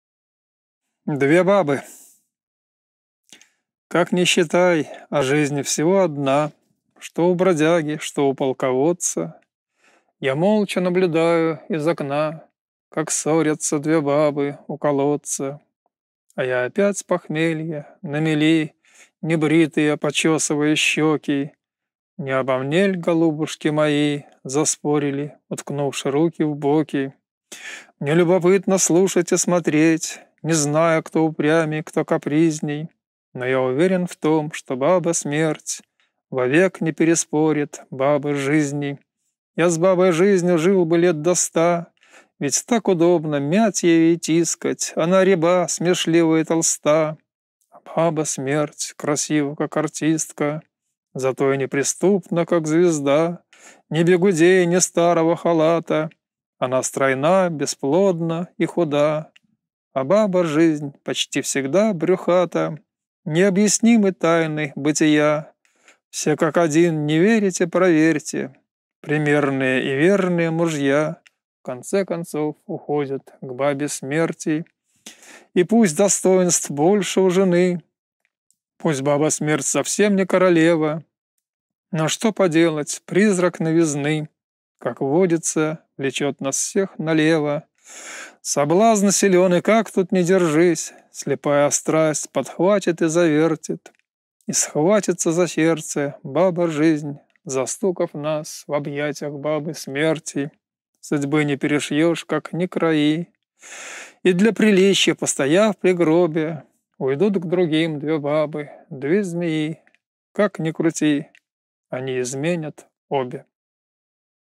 Владислав Артёмов. Две бабы
Запись июль 2017 г.

ДВЕ БАБЫ

Как ни считай, а жизнь всего одна,
Что у бродяги, что у полководца…
Я молча наблюдаю из окна,
Как ссорятся две бабы у колодца.

А я опять с похмелья на мели,
Небритые почёсываю щёки,
Не обо мне ль, голубушки мои,
Заспорили, уткнувши руки в боки…

Мне любопытно слушать и смотреть,
Не знаю, кто упрямей, кто капризней,
Но я уверен в том, что баба-смерть
Вовек не переспорит бабы-жизни.

С обеими связался, сам не рад,
Давно бы я прогнал и ту, и эту,
Но, к сожаленью, выбор небогат,
Сказать по правде, выбора и нету…

Я с бабой-жизнью жил бы лет до ста,
Ведь так удобно мять её и тискать,
Она – ряба, смешлива и толста…
А баба-смерть – красива, как артистка!..

Зато и неприступна, как звезда,
Ни бигудей, ни старого халата,
Она стройна, бесплодна и худа…
А баба-жизнь почти всегда – брюхата.

Необъяснимы тайны бытия,
Все как один, – не верите, проверьте,
Примерные и верные мужья
В конце концов, уходят к бабе-смерти.

И пусть достоинств больше у жены,
Пусть баба-смерть – совсем не королева,
Но что поделать – призрак новизны,
Как водится, влечёт нас всех налево.

Соблазн силён и как тут ни держись,
Слепая страсть подхватит и завертит…
И схватится за сердце баба-жизнь,
Застукав нас в объятьях бабы-смерти.

Судьбы не перешьёшь, как ни крои,
И для приличья постояв при гробе,
Уйдут к другим две бабы, две змеи,
Уж извини, они изменят – обе!..